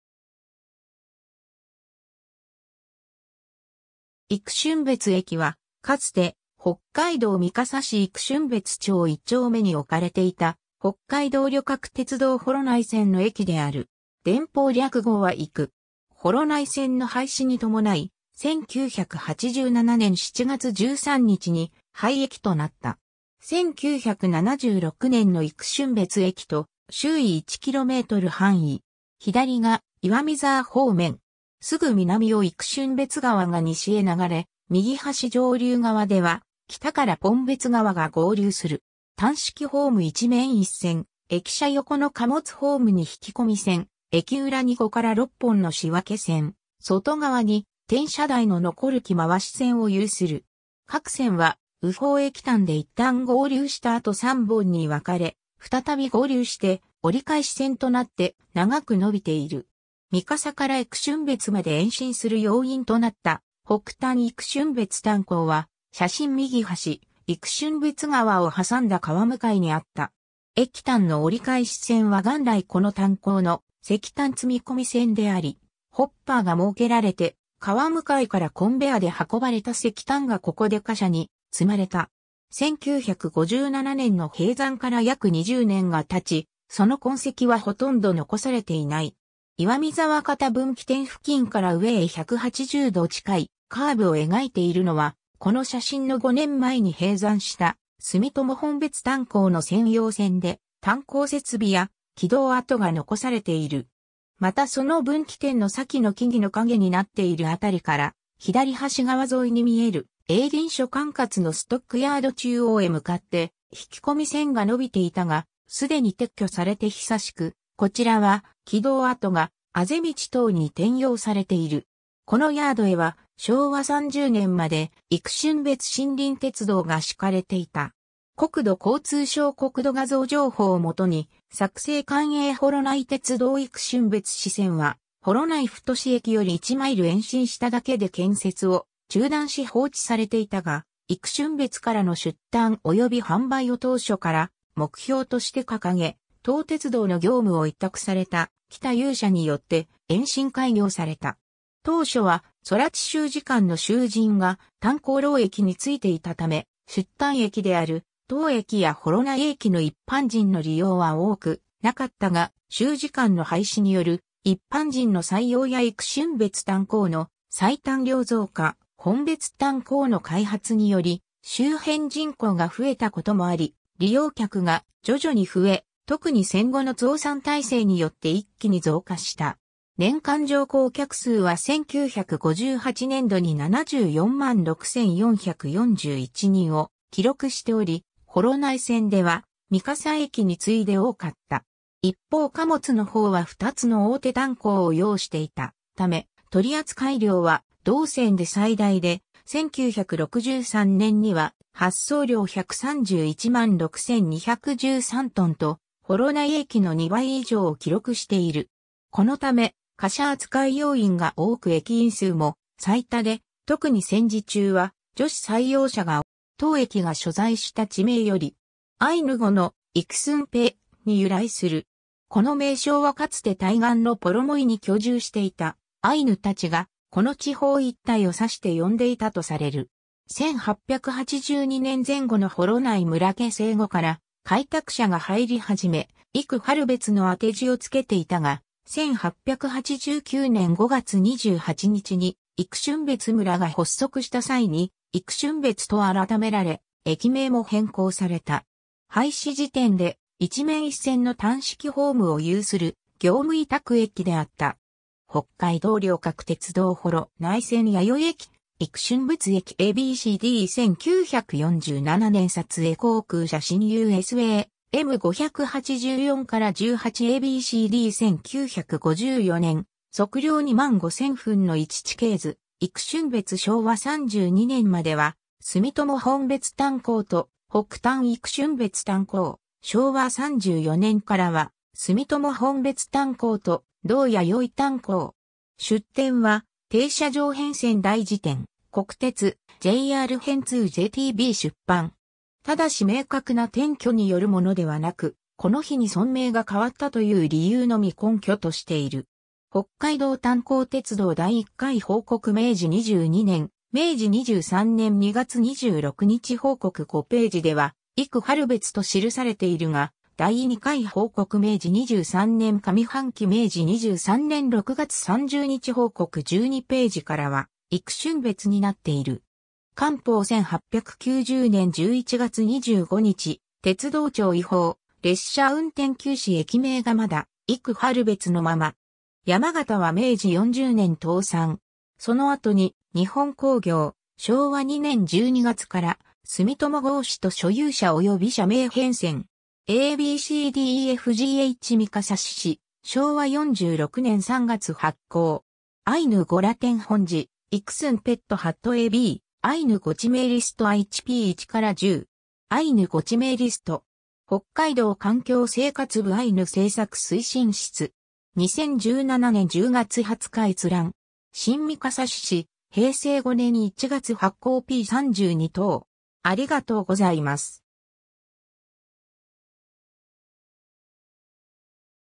幾春別駅
幾春別駅（いくしゅんべつえき）は、かつて北海道三笠市幾春別町1丁目に置かれていた、北海道旅客鉄道（JR北海道）幌内線の駅である。
電報略号はイク。
幌内線の廃止に伴い、1987年（昭和62年）7月13日に廃駅となった。
 1976年の幾春別駅と周囲1km範囲。
左が岩見沢方面。
すぐ南を幾春別川が西へ流れ、右端上流側では北から奔別川が合流する。
単式ホーム1面1線、駅舎横の貨物ホームに引込み線、駅裏に5-6本の仕分線、外側に転車台の残る機回し線を有する。
各線は右方駅端で一旦合流した後3本に分かれ、再び合流して折り返し線となって長く伸びている。
 三笠から幾春別まで延伸する要因となった北炭幾春別炭鉱は、写真右端、幾春別川を挟んだ川向にあった。
駅端の折り返し線は元来この炭鉱の石炭積み込み線であり、ホッパーが設けられて、川向からコンベアで運ばれた石炭がここで貨車に積まれた。
1957年（昭和32年）の閉山から約20年が経ち、その痕跡は殆ど残されていない。
岩見沢方分岐点付近から上へ180°近いカーブを描いているのは、この写真の5年前に閉山した住友奔別炭鉱の専用線で、炭鉱設備や軌道跡が残されている。
またその分岐点の先の木々の影になっている辺りから左端川沿いに見える営林署管轄のストックヤード（土場）中央へ向かって引込み線が伸びていたが、既に撤去されて久しく、こちらは軌道跡が畦道等に転用されている。
このヤードへは昭和30年まで幾春別森林鉄道が敷かれていた。
国土交通省 国土画像情報（カラー空中写真）を基に作成 官営幌内鉄道幾春別支線は、幌内太駅（三笠駅）より1マイル延伸しただけで建設を中断し放置されていたが、幾春別からの出炭及び販売を当初から目標として掲げ、当鉄道の業務を委託された北有社によって延伸開業された。
当初は空知集治監の囚人が炭鉱労役に就いていたため、出炭駅である当駅や幌内駅の一般人の利用は多くなかったが、集治監の廃止による一般人の採用や幾春別炭砿の採炭量増加、奔別炭砿の開発により周辺人口が増えたこともあり、利用客が徐々に増え 、特に戦後の増産体制によって一気に増加した。
年間乗降客数は1958年度（昭和33年度）に746,441人を記録しており、幌内線では三笠駅に次いで多かった。
一方貨物の方は2つの大手炭鉱を擁していたため取扱量は同線で最大で、1963年（昭和38年）には発送量1,316,213tと幌内駅の2倍以上を記録している。
このため貨車扱い要員が多く駅員数も最多で、特に戦時中は女子採用者が多く、重労働であったためか、1942年（昭和17年）には70人という大所帯であった。
 当駅が所在した地名より。
アイヌ語の「イクスンペッ」（向こう側にある・川）に由来する。
 この名称はかつて対岸のポロモイ（幌向）に居住していたアイヌ達が、この地方一帯を指して呼んでいたとされる。
1882年（明治15年）前後の幌内村形成後から開拓者が入り始め、「郁春別」の当て字を付けていたが、1889年（明治22年）5月28日に幾春別村が発足した際に「幾春別」と改められ、駅名も変更された。
 廃止時点で、1面1線の単式ホームを有する業務委託駅であった。
 北海道旅客鉄道（JR北海道） 幌内線 弥生駅 - 幾春別駅 ^ a b c d 1947年撮影航空写真 USA-M584-18（国土地理院 地図・空中写真閲覧サービス） ^ a b c d 1954年測量2万5千分の1地形図「幾春別」 ^ 昭和32年までは住友奔別炭砿と北炭幾春別炭砿、昭和34年からは住友奔別炭砿と同弥生炭砿。
 ^ 出典は「停車場変遷大事典 国鉄・JR編II JTB出版」。
ただし明確な典拠によるものではなく、この日に村名が変わったという理由のみ根拠としている。
「北海道炭礦鐡道 第一回報告 明治22年　明治23年2月26日報告」5頁では「郁春別」と記されているが、「第二回報告 明治23年上半期　明治23年6月30日報告」12頁からは「幾春別」になっている。
 ^ 『官報』 1890年11月25日　鉄道庁彙報「列車運転休止」（国立国会図書館デジタルコレクション）　駅名がまだ「郁春別」のまま。
 ^ 山県は明治40年倒産。
その後に日本興業（奔別炭礦/山下鉱業/山下汽船鉱業）、昭和2年12月から住友合資（住友鉱業/井...